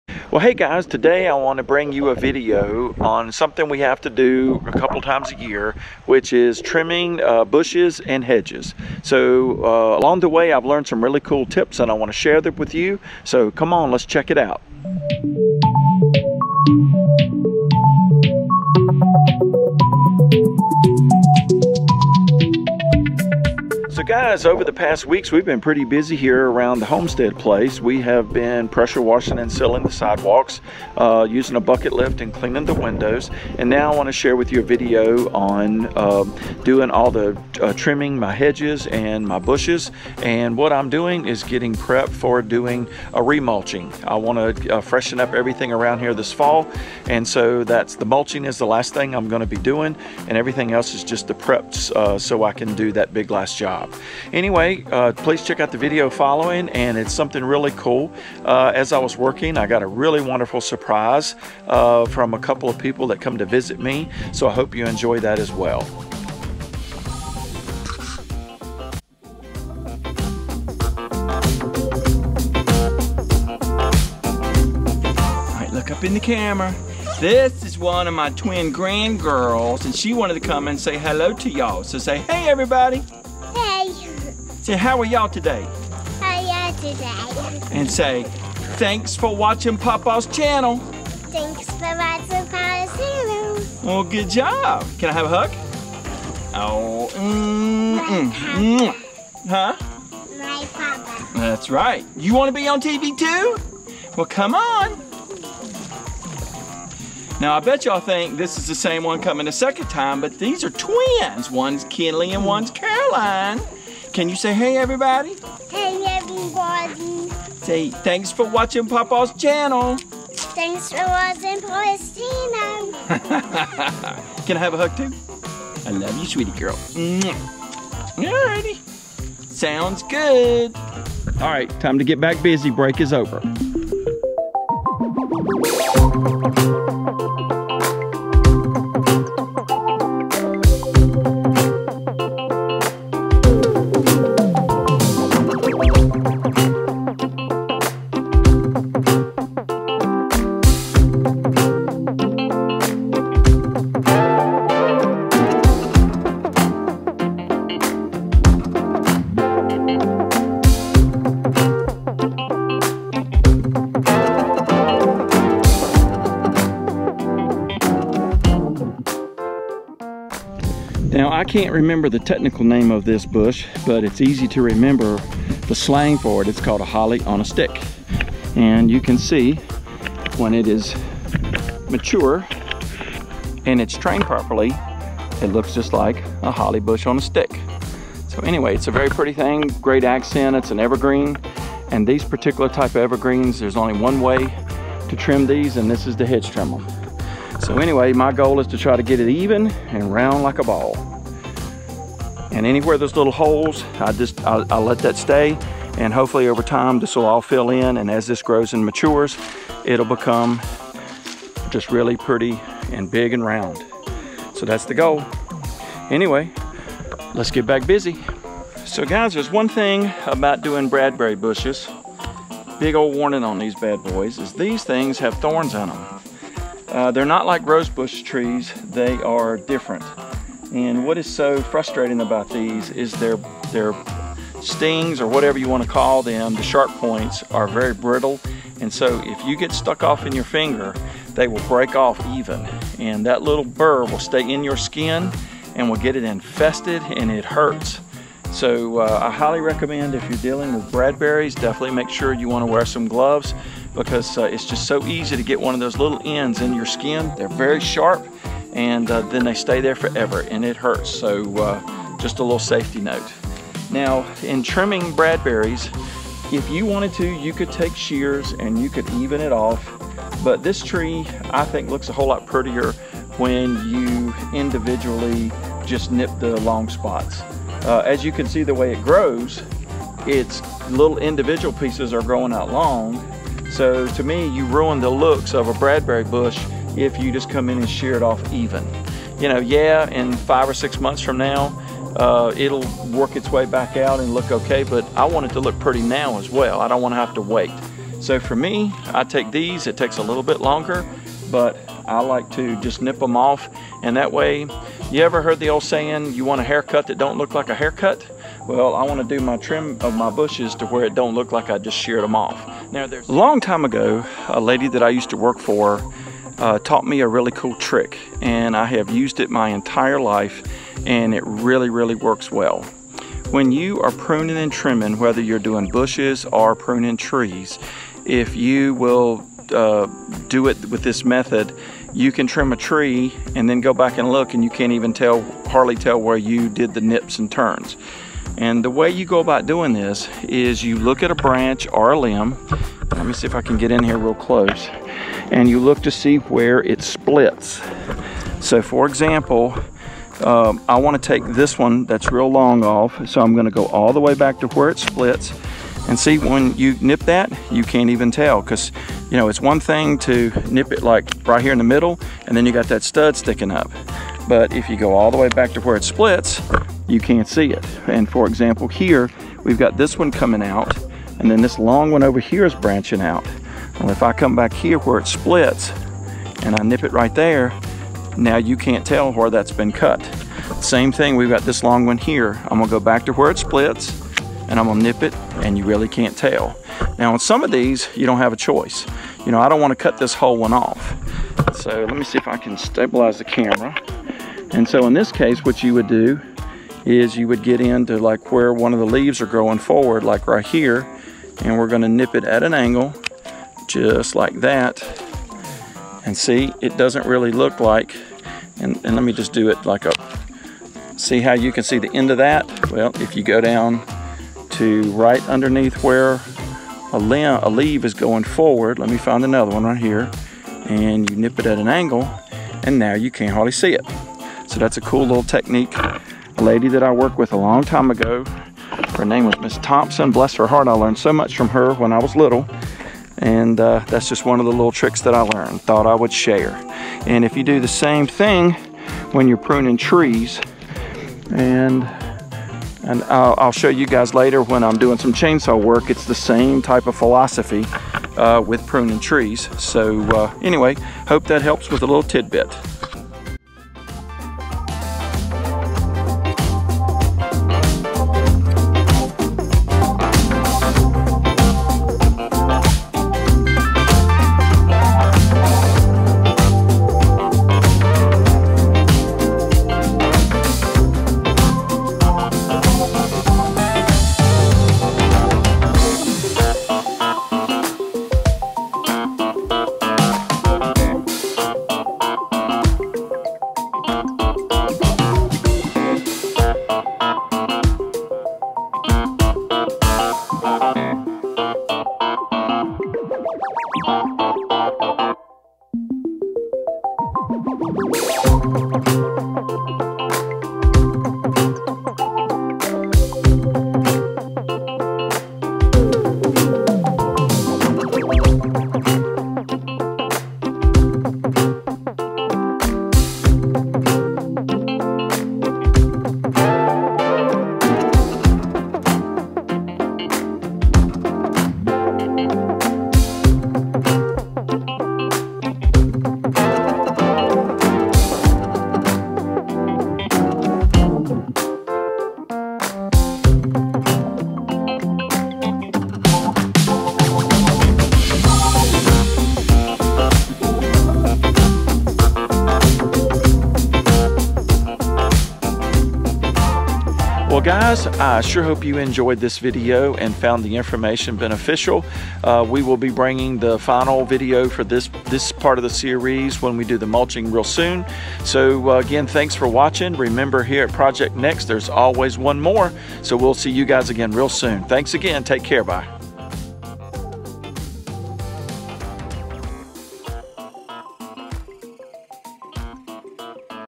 MY LANDSCAPE DESIGNS / TRIMMING HEDGES & BUSHES
LANDSCAPING MULCHING / TRIMMING HEDGES & BUSHES / Hey Check out my video on trimming Hedges and Bushes. I'll share Tech. Tips that I learned years ago that have always worked GREAT !